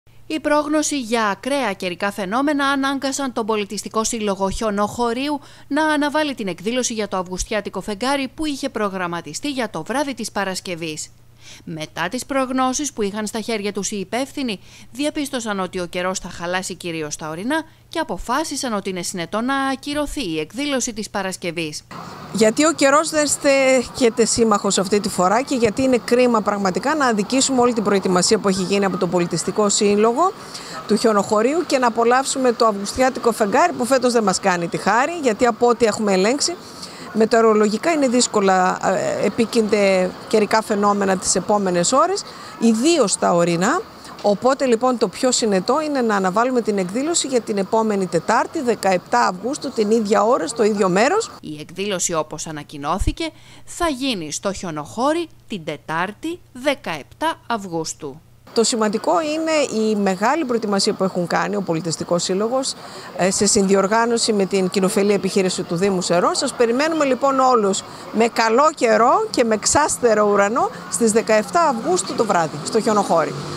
Στο Χιονοχώρι αύριο η εκδήλωση για την πανσέληνο
Η πρόγνωση για τα ακραία καιρικά φαινόμενα ανάγκασαν τον Πολιτιστικό Σύλλογο Χιονοχωρίου να αναβάλλει την εκδήλωση για το Αυγουστιάτικο Φεγγάρι που είχε προγραμματιστεί για το βράδυ της Παρασκευής.